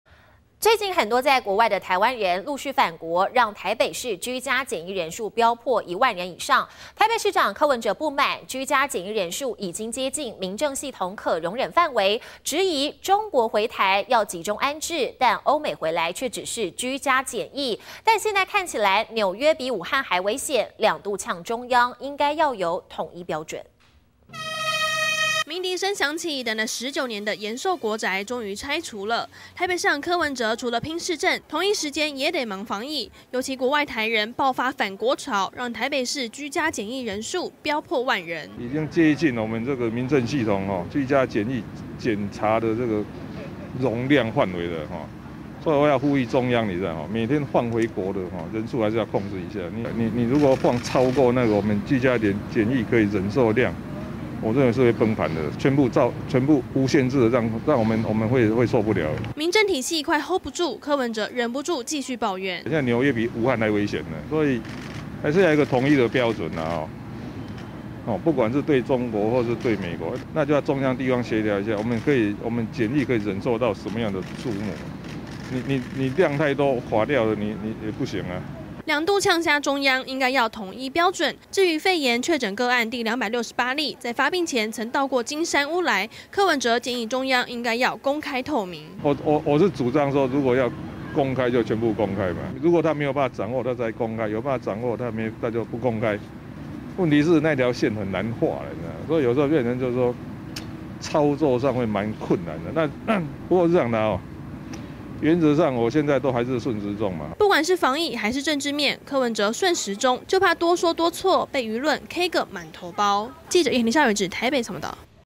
"紐約比武漢危險" 柯:返國檢疫應統一標準
武漢肺炎在台灣的確診個案，快逼近300例，尤其最近很多在國外的台灣人陸續返國，讓台北市居家檢疫人數，飆破1萬人以上，台北市長柯文哲不滿，居家檢疫人數已接近民政系統可容忍範圍，質疑中國回台要集中安置、歐美回來卻只是居家檢疫，但現在看起來「紐約比武漢還危險」，兩度嗆中央應該要有統一標準。